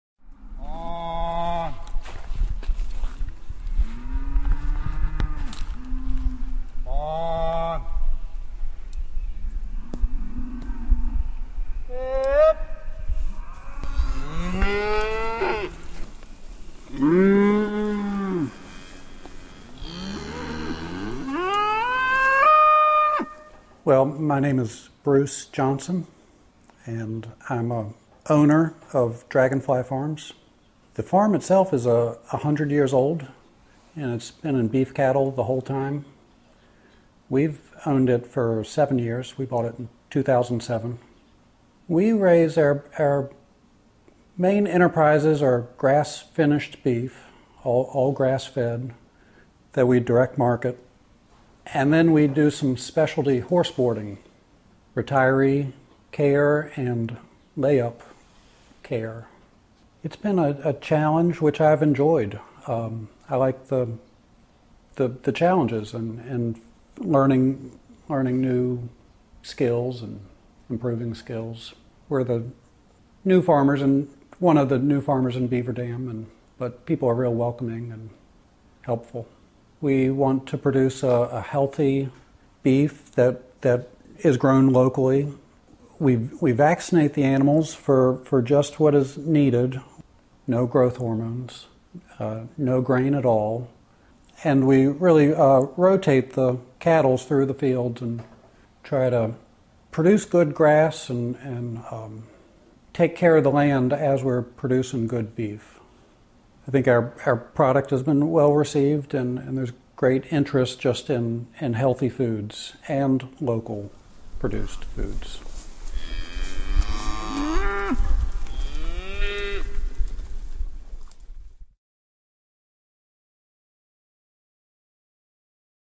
Dragonfly Farms
Bruce Johnson, owner of Dragonfly Farms in Beaverdam, Va., talks about his approach to raising all-natural, grass-fed beef.